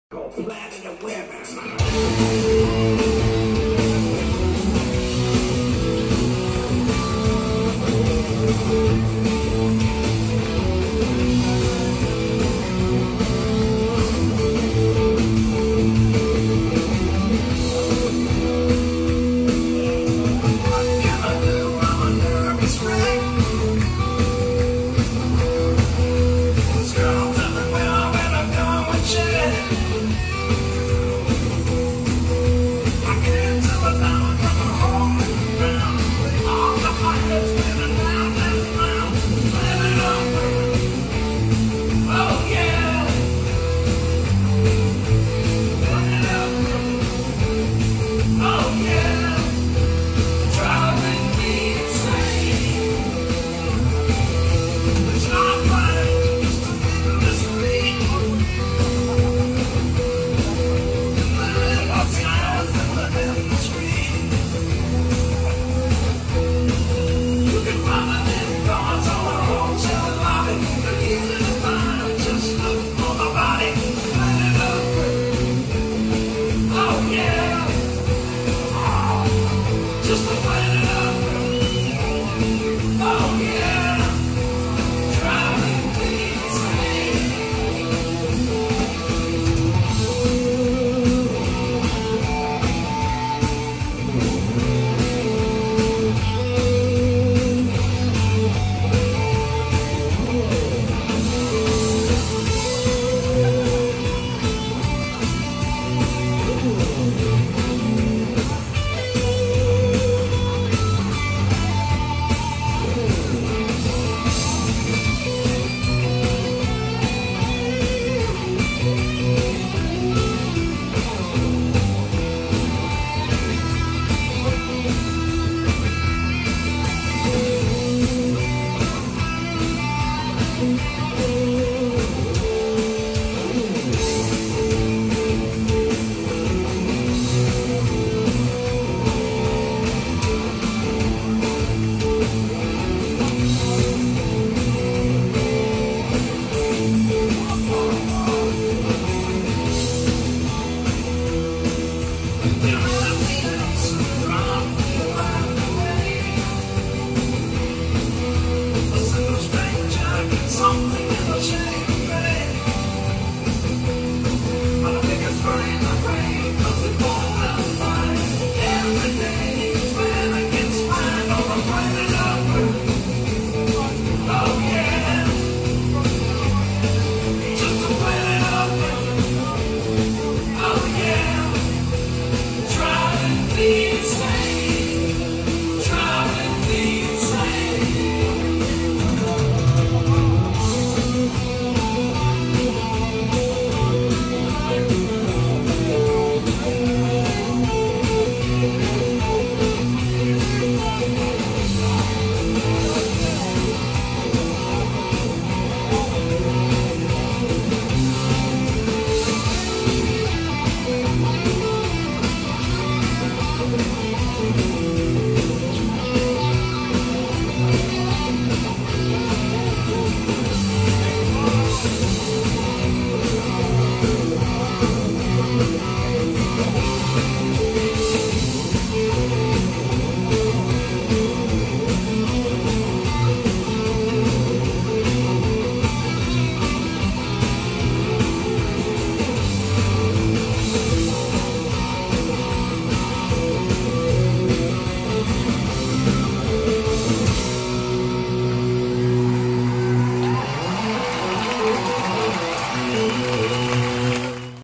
Chainsaw artist,buzz saws music by zz top recorded in vermont
Chain Saw artist doing their craft in Chester Vermont. First Annual event.